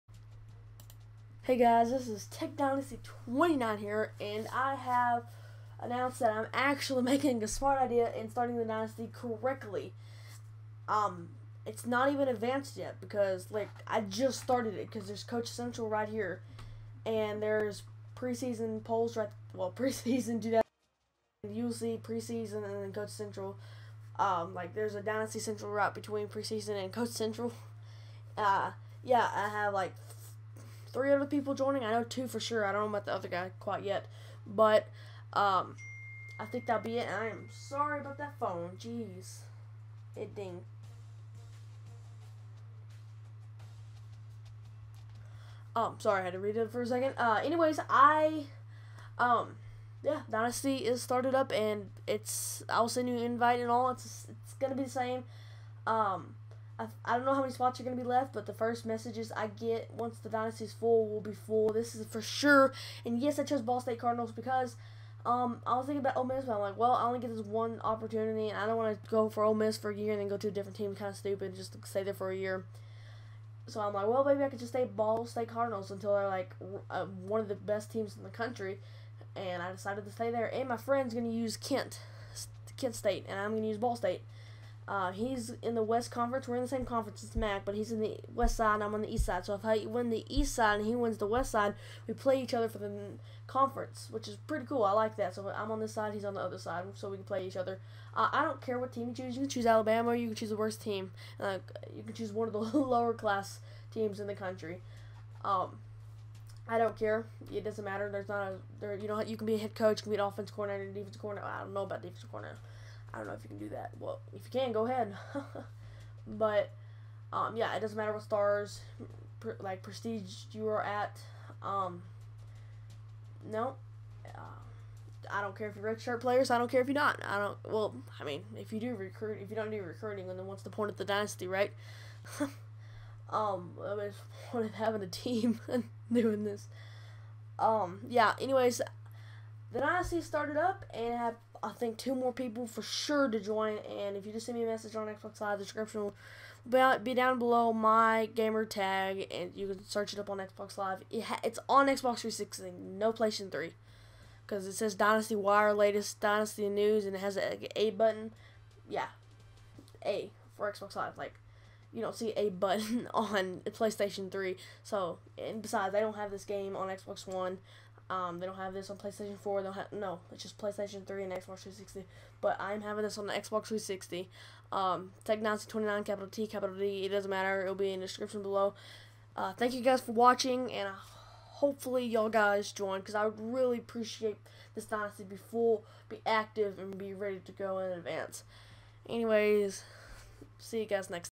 IT'S FOR SURE!
I chose Ball State and the Dynasty is ready to go! My gamertag is TechDynasty29